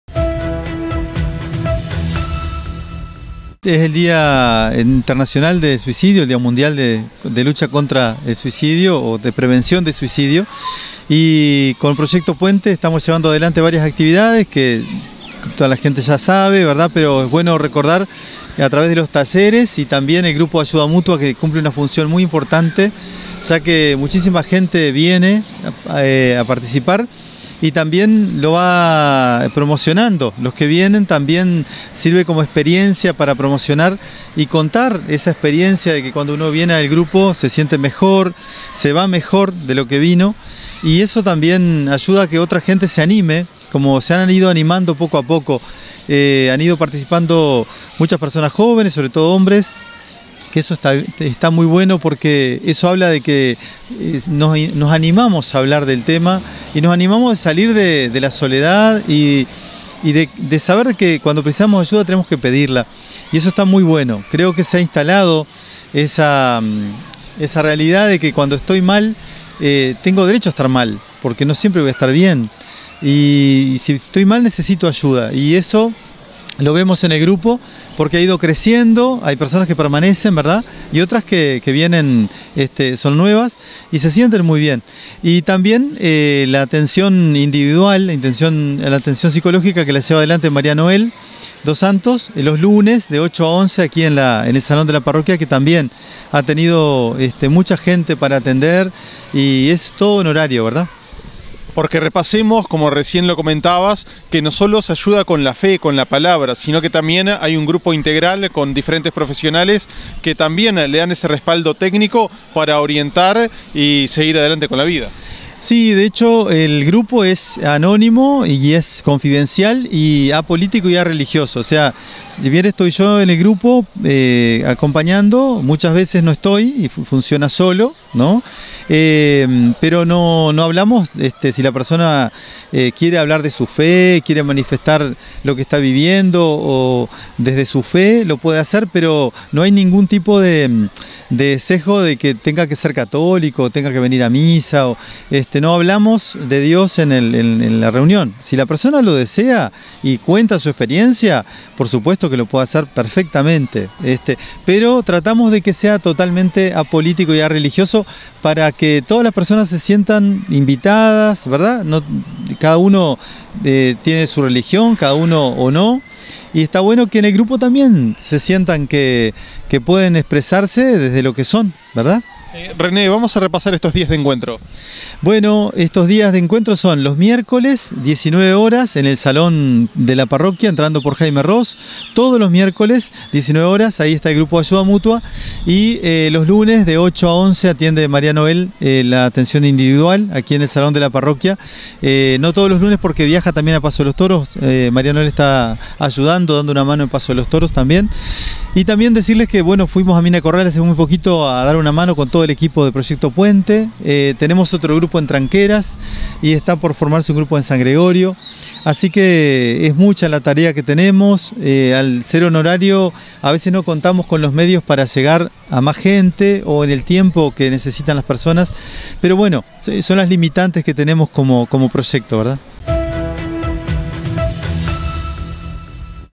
El domingo es el Día de Prevención del Suicidio
Desde Parroquia Santa Cruz y el Proyecto Puente están recorriendo el departamento para combatir la problemática. Hablamos con el Padre René Da Silva